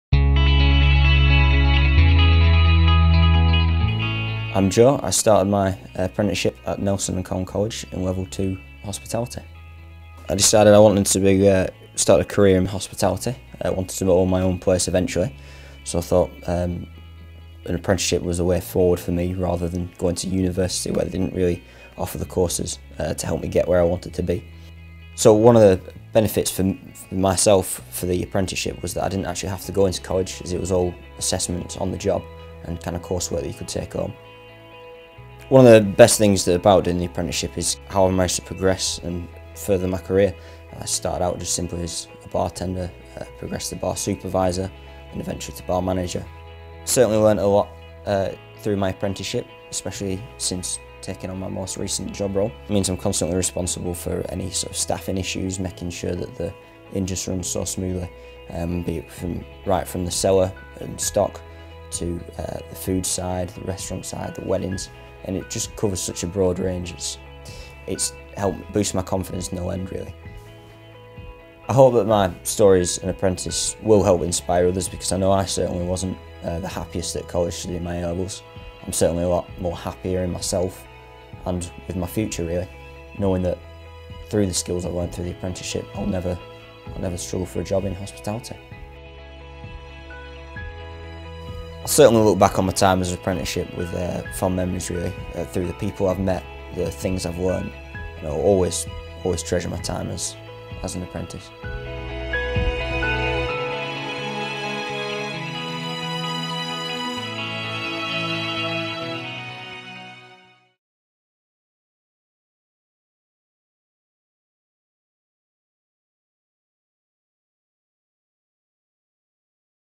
Joe's Hospitality Apprenticeship story
Joe talks about the skills he has learnt as an apprentice and why he would encourage others to take on an apprenticeship...